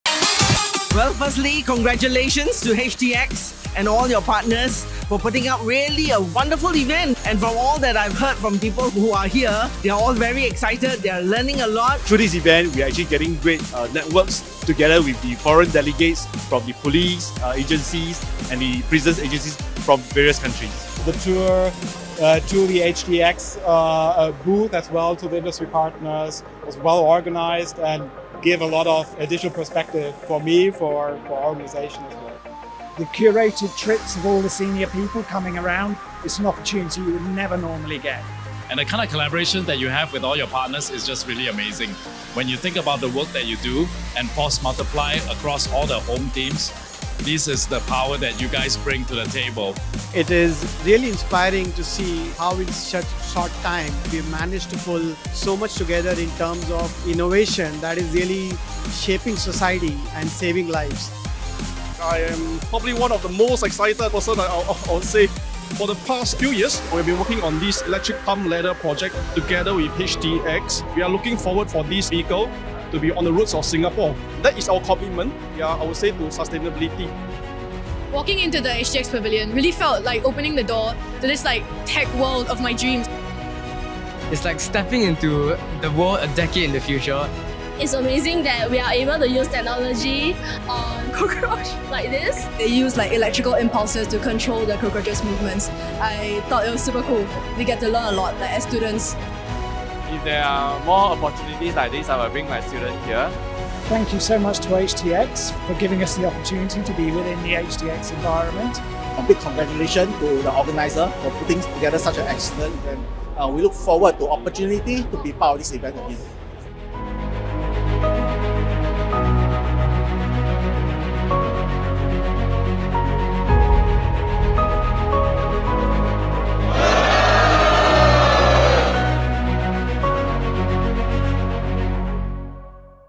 Milipol Asia Pacific - TechX Summit 2024 | Highlights from the Exhibition
The 3-day MAP-TXS was jam-packed with activities, and attendees and curious visitors filled the exhibition and conference halls at the Marina Bay Sands convention centre. Check out some highlights from the video and read